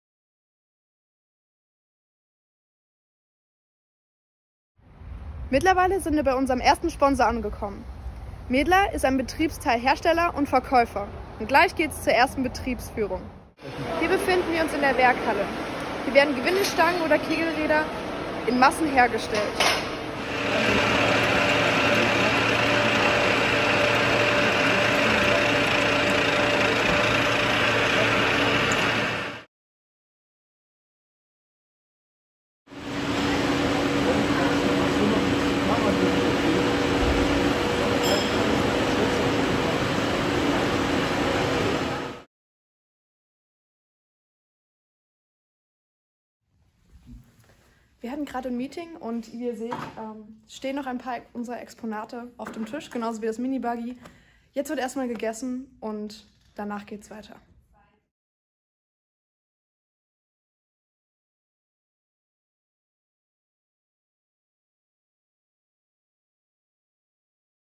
221028 Maedler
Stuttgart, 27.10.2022
Besuch bei Mädler mit Meeting und Rundgang in der Produktion.
Video by Cosma Heckel